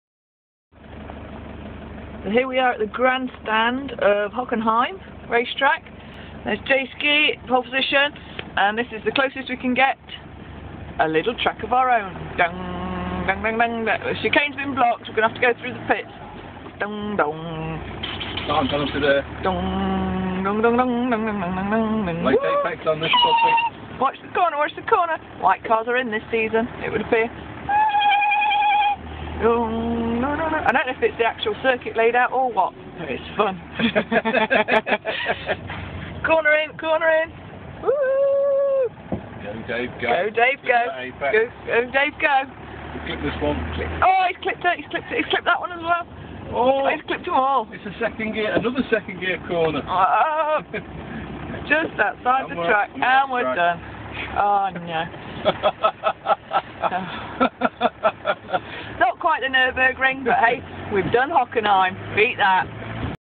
OurTour Dave the Motorhome Does Hockenheim, Sort Of...
Dave the motorhome takes on a mini racetrack alongside the mighty Hockenheim circuit! For more details and photos of our tour of Europe in a motorhome, please see our website OurTour.co.uk.